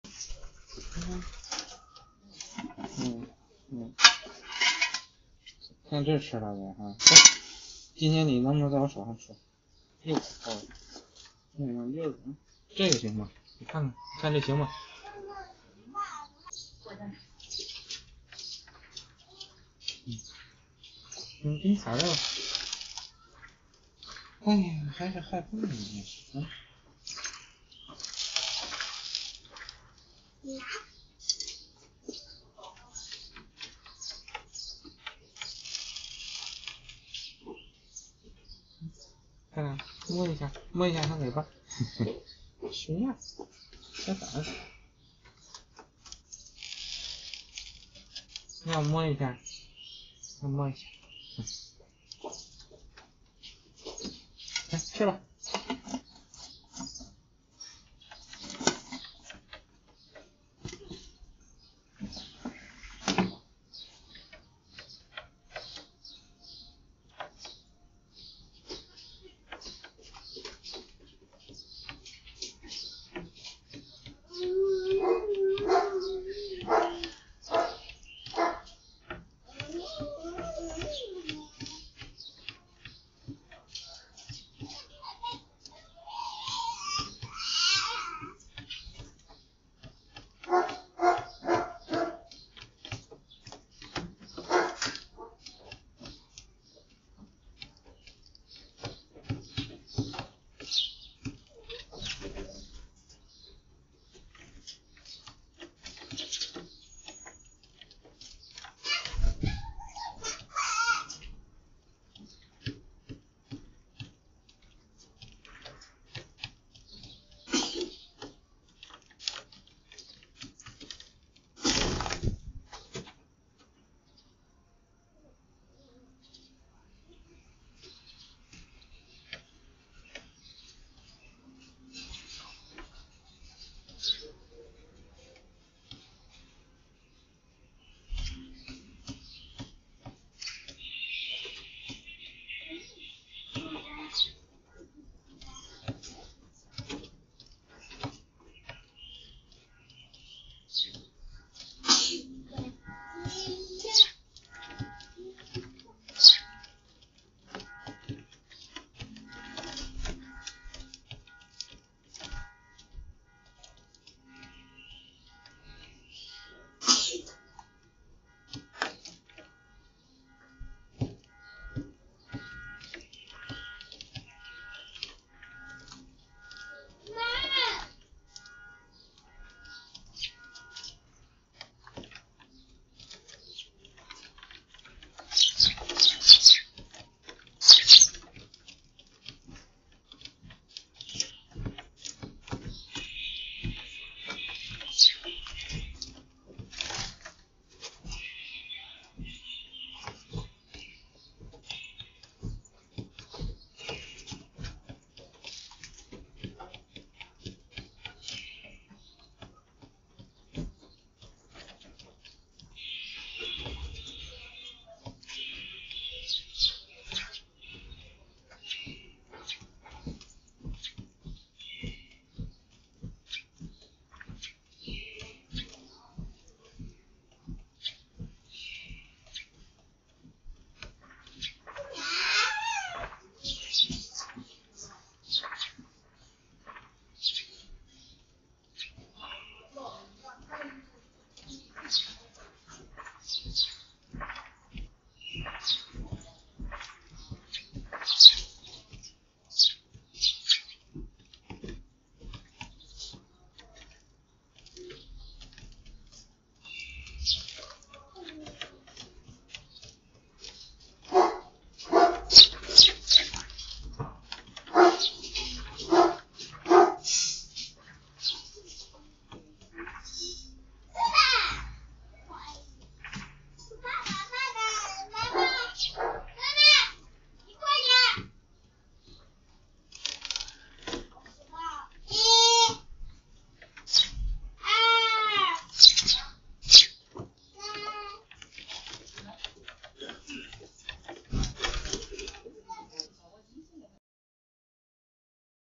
spotted Dove & Turtledove
On the 8th day of National Day, the turtledoves still love us, we come home, they also come